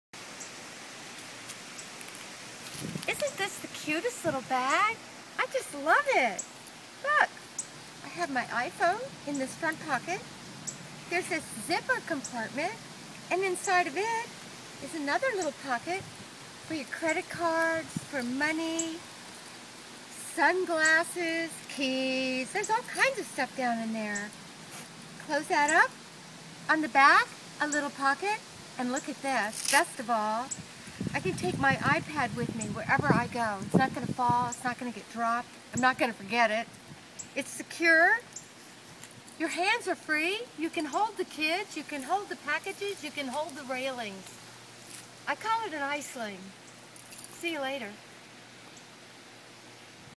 Introducing the i-Sling Bag
This short video introduces the fabulous new i-Sling tote!